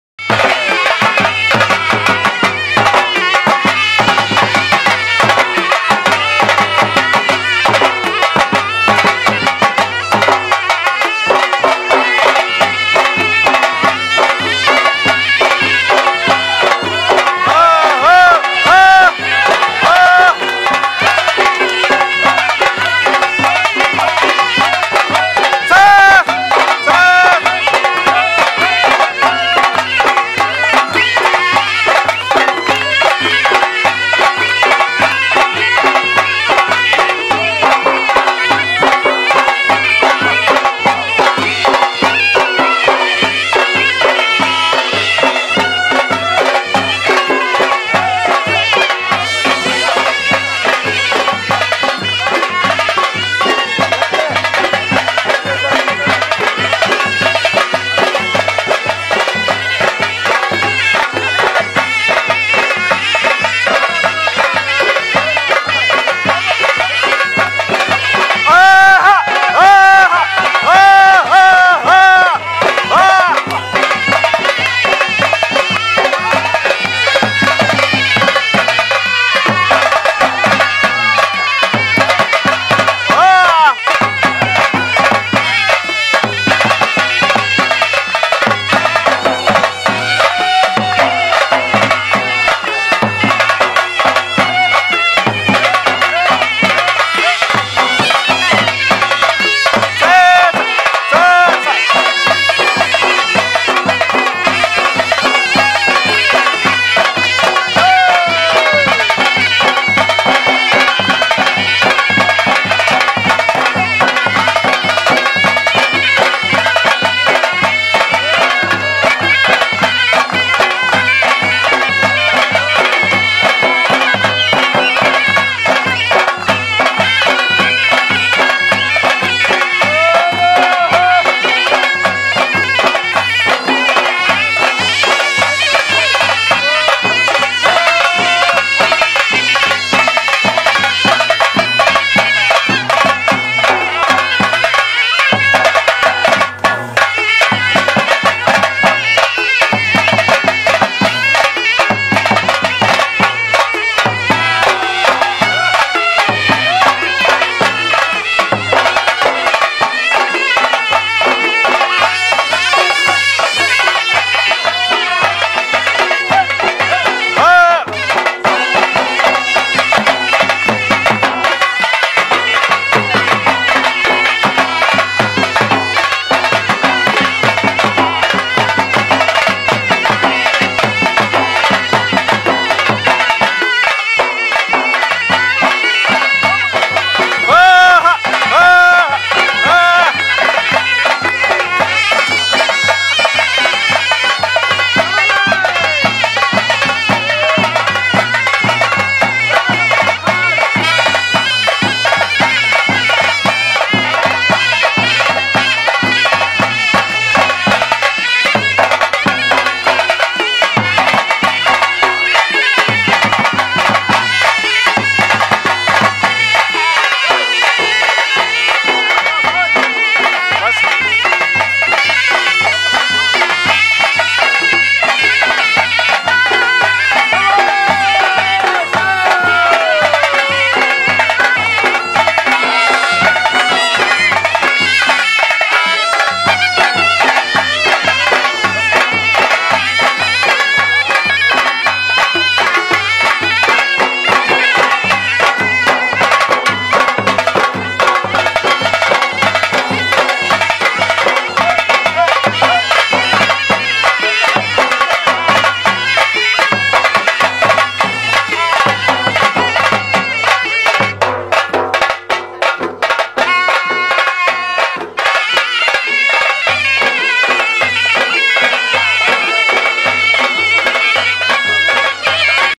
Wedding in Hunza Valley ||Hunza culture and traditional wedding
Hunza Gilgit Baltistan Traditional Dance the

culture heritage of the Gilgit Baltistan is a rich and unique culture all around the world